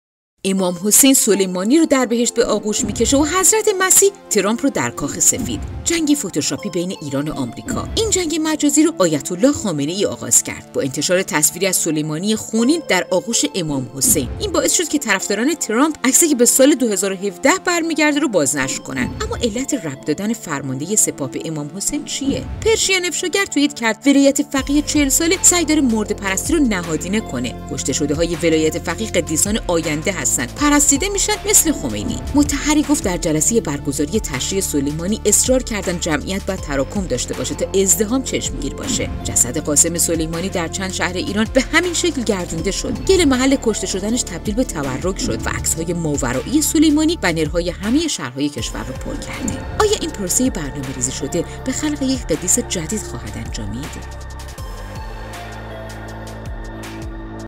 تلاش برای قدیس سازی
به نظر شما حکومت چقدر در رشد خرافات مرده پرستی تو کشورمون تأثیر داشته؟